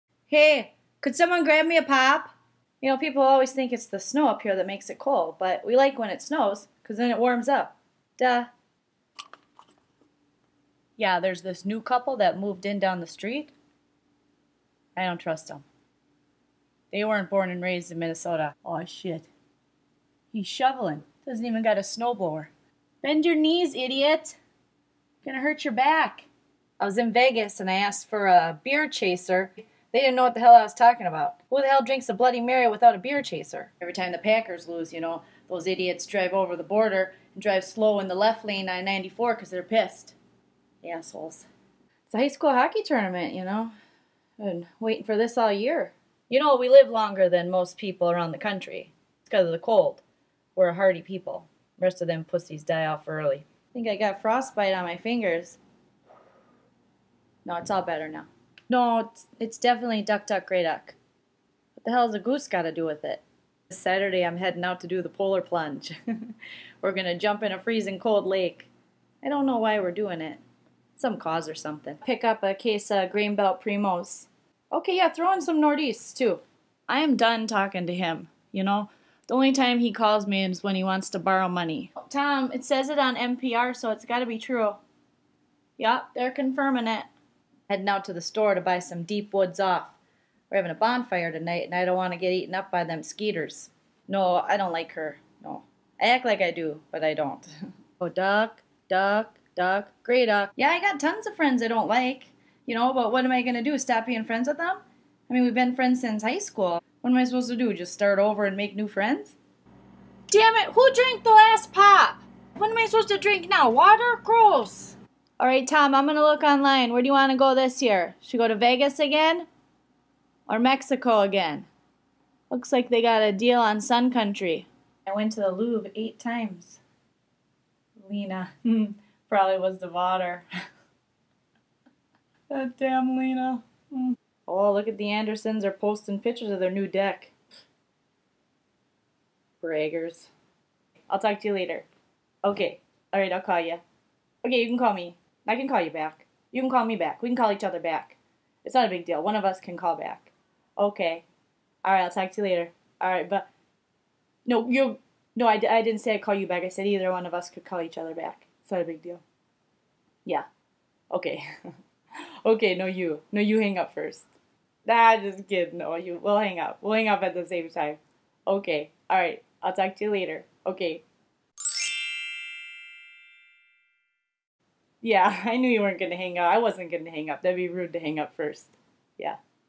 How to Talk Like a Minnesotan | Comedian Joleen Lunzer | Oh Fer Cute! | Ope | Hotdish | You Betcha
Ever wondered how to speak like a Minnesotan? Well, here is how! This is a follow-up to my first Minnesota accent video: Sh*t Minnesota Gals Say